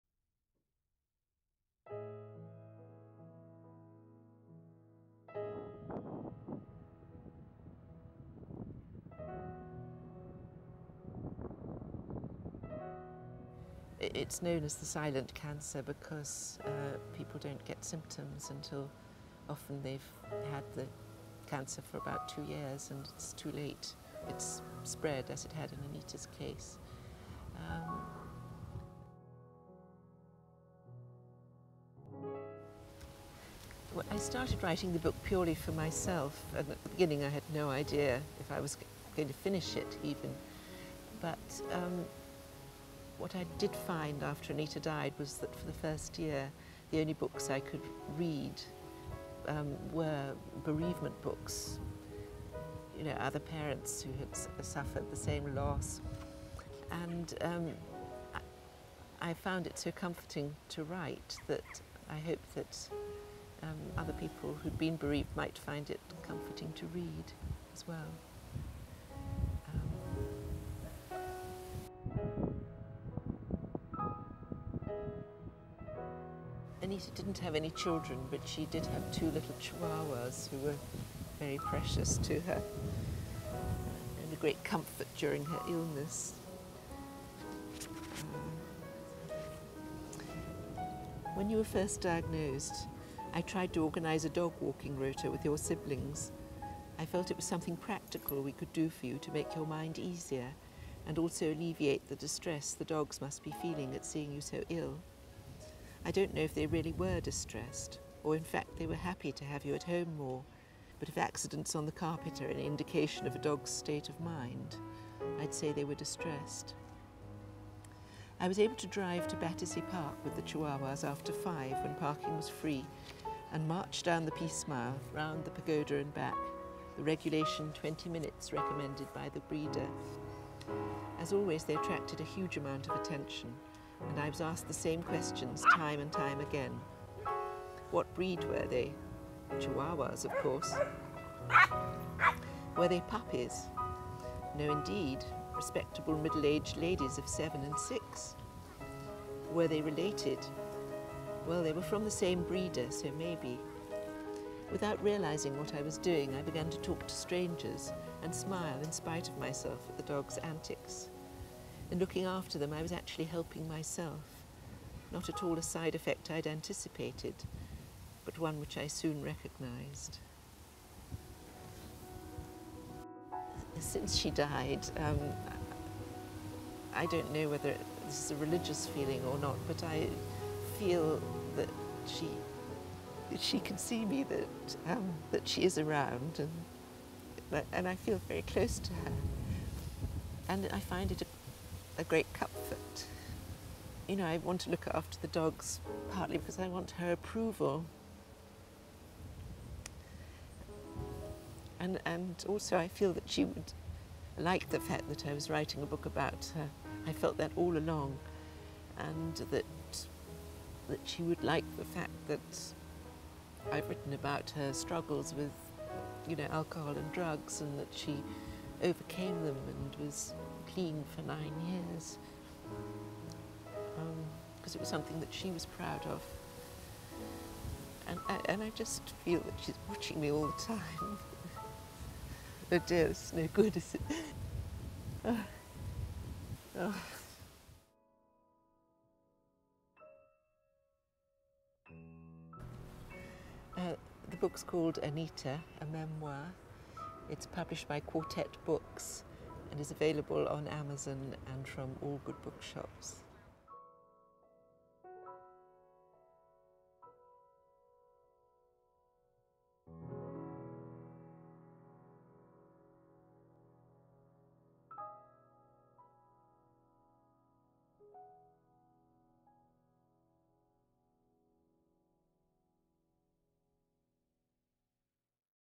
Anita - A Memoir by Anne Dunhill
"Shortly before going into hospital, Anita asked me, almost playfully, what I'd do if she died. Taken aback, I allowed myself, for once, to be spontaneous. 'Oh, it'd be the end of me,' I said. At this, she got very distressed and made me promise I'd keep going after her death, so of course I did. Since she died I've sometimes asked myself what on earth the point is of me remaining here once my child has gone, but my thought always come back to this conversation, and I feel I should stick around as promised, even if I don't know why." - Anne Dunhill.